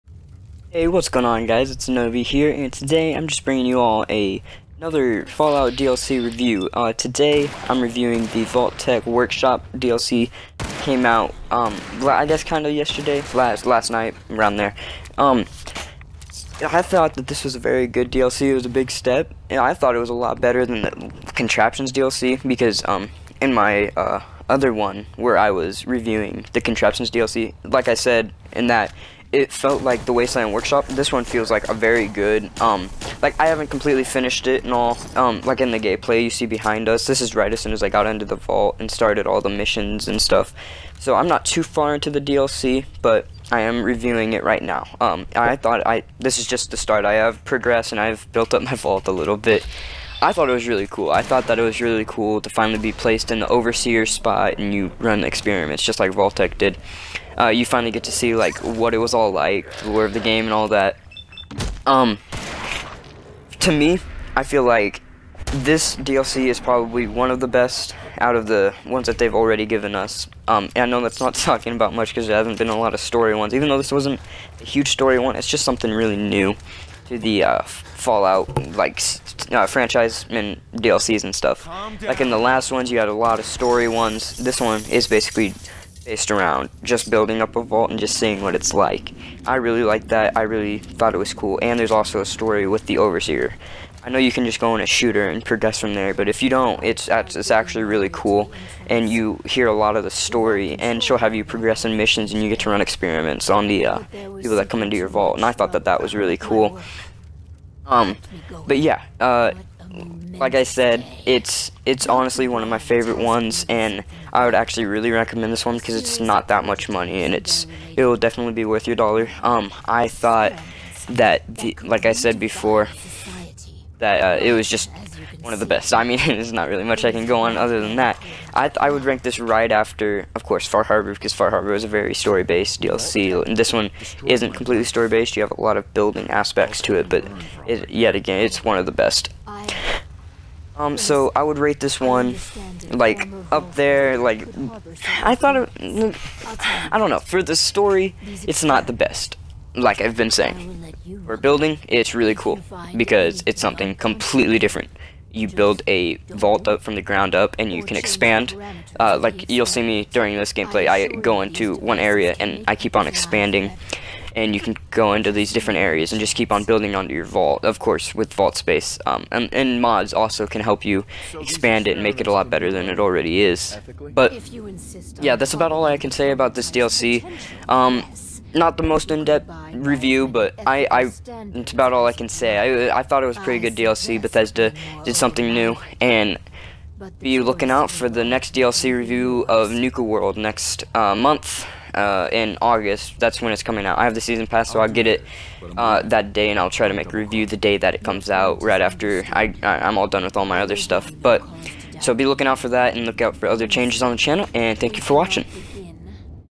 IS IT WORTH THE MONEY?? FALLOUT 4 VAULT-TECH WORKSHOP REVIEW!!
Hey guys welcome to episode two of "Is it worth the money??" we just talked about my opinion of the new Fallout 4 DLC!